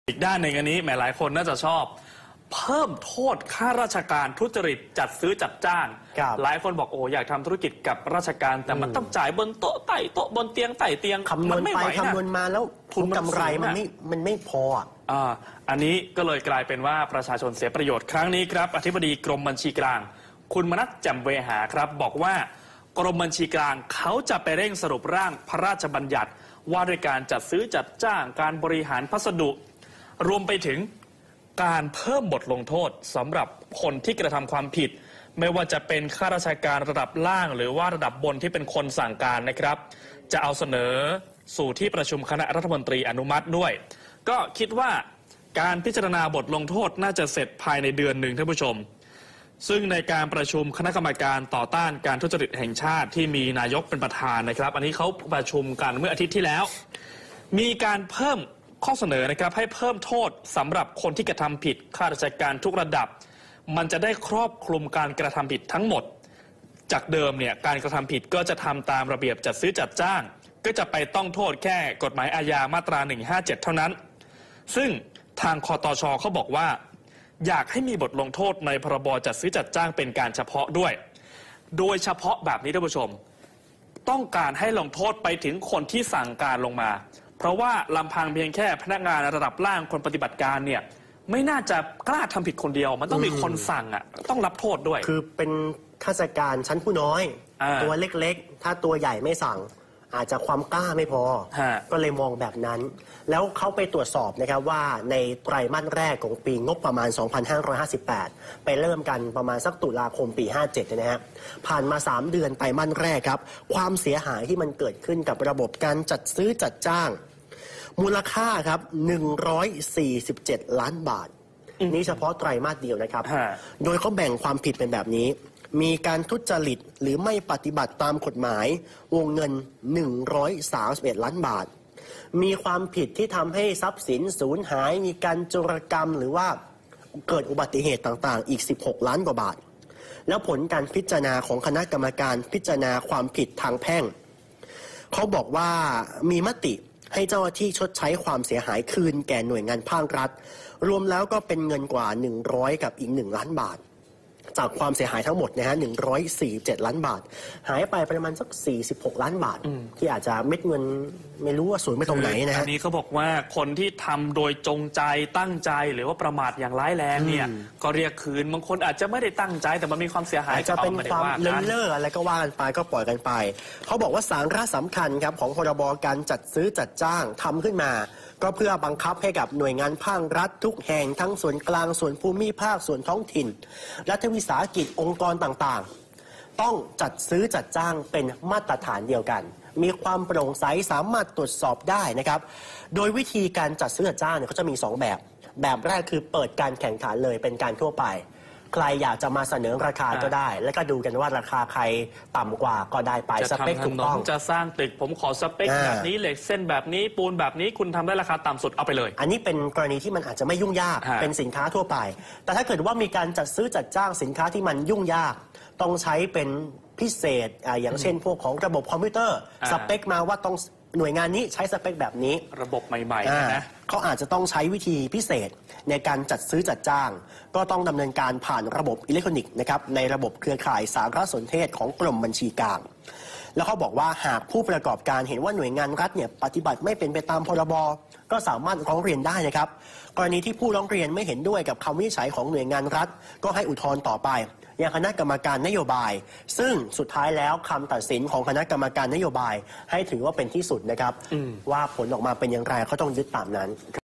Now 26 : เพิ่มโทษข้าราชการทุจริตจัดซื้อจัดจ้าง 20/1/2558
นายมนัส แจ่มเวหา อธิบดีกรมบัญชีกลางเปิดเผยว่า กรมบัญชีกลางจะเร่งสรุปร่างพระราชบัญญัติ (พ.ร.บ.) ว่าด้วยการจัดซื้อจัดจ้างและการบริหารพัสดุ ซึ่งรวมถึง การเพิ่มบทลงโทษสำหรับผู้กระทำผิดทั้งในส่วนข้าราชการระดับล่างไปจนถึงระดับบนที่เป็นผู้สั่งการ เพื่อเสนอเข้าสู่ที่ประชุมคณะรัฐมนตรีอนุมัติ โดยคาดว่าการพิจารณาบทลงโทษจะแล้วเสร็จภายใน 1 เดือน
ทั้งนี้ ในการประชุมคณะกรรมการต่อต้านการทุจริตแห่งชาติ(คตช.)ที่มีพลเอกประยุทธ์ จันทร์โอชา นายกรัฐมนตรี เป็นประธานเมื่อสัปดาห์ที่ผ่านมา ได้มีข้อเสนอให้เพิ่มโทษสำหรับผู้กระทำผิด ข้าราชการทุกระดับ เพื่อให้บทลงโทษนั้นคลอบคลุมการกระทำความผิด จากเดิมการกระทำผิดตามระเบียบจัดซื้อจัดจ้างจะต้องโทษกฎหมายอาญามาตรา 157 เท่านั้น
(เพลทคำพูด)“คตช. ต้องการให้มีบทลงโทษในพ.ร.บ.จัดซื้อจัดจ้างฯ เป็นการเฉพาะ โดยเฉพาะต้องการให้ลงโทษไปถึงผู้สั่งการ เพราะลำพังเพียงพนักงานระดับล่างคงไม่กล้าทำผิดเพียงคนเดียว”
ทั้งนี้ ในช่วงไตรมาสแรกปีงบ 2558 (ตุลาคม-ธันวาคม 2557) เกิดความเสียหายเกี่ยวกับการจัดซื้อจัดจ้างที่ได้ผ่านการพิจารณาของคณะกรรมการพิจารณาความรับผิดทางแพ่ง 92 สำนวน มูลค่าความเสียหาย 147 ล้านบาท โดยแยกความเสียหายออกเป็น
(กราฟฟิค) ความผิดจัดซื้อจัดจ้างไตรมาส1/58
1 ทุจริตหรือไม่ปฎิบัติตามกม. 131 ล้านบาท
2 ทรัพย์สินสูญหาย,โจรกรรม,เกิดอุบัติเหตุ 16 ล้านบาท
ทั้งนี้ ผลการพิจารณาได้มีมติให้เจ้าหน้าที่ชดใช้ความเสียหายคืนแก่หน่วยงานของรัฐเป็นเงินกว่า 101 ล้านบาท โดยจะเรียกชดใช้เงินต่อไป ซึ่งกรณีที่เรียกชดใช้จะเป็นกรณีที่กระทำโดยจงใจ หรือประมาทเลินเล่ออย่างร้ายแรงเท่านั้น
สาระสำคัญของพ.ร.บ.จัดซื้อจัดจ้างฯ ทำขึ้น เพื่อนำมาบังคับใช้กับหน่วยงานภาครัฐทุกแห่ง ทั้งส่วนกลางและส่วนภูมิภาค และส่วนท้องถิ่น รวมถึง รัฐวิสาหกิจตามกฎหมายว่าด้วยวิธีการงบประมาณ องค์กรตามรัฐธรรมนูญ องค์กรมหาชน และหน่วยงานอิสระของรัฐ ซึ่งทำให้การจัดซื้อจัดจ้างเป็นมาตรฐานเดียวกันโปร่งใส ตรวจสอบได้